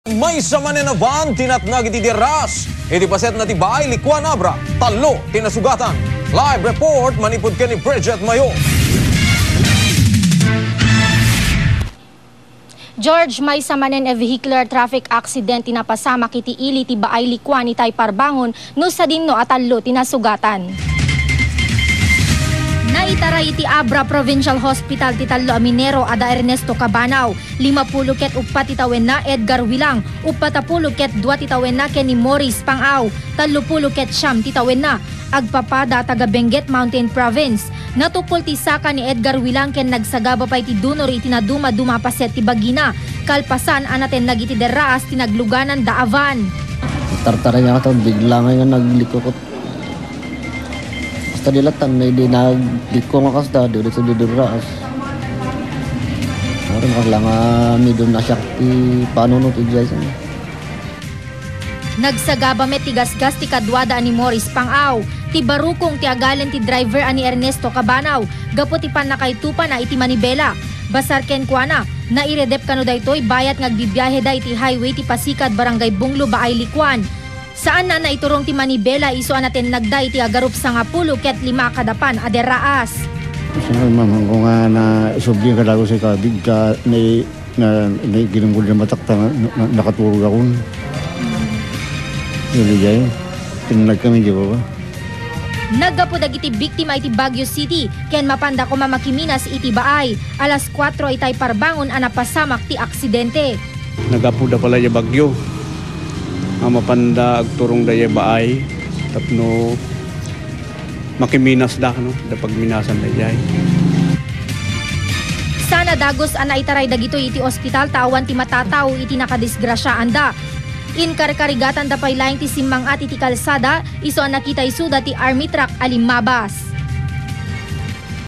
BAAY-LICUAN, ABRA - Maysa manen a Van, natnag iti derraas iti Baay-Licuan.
BALITANG ILOKANO(BAAY-LICUAN, ABRA) - Maysa manen a Van, natnag iti derraas iti Baay-Licuan.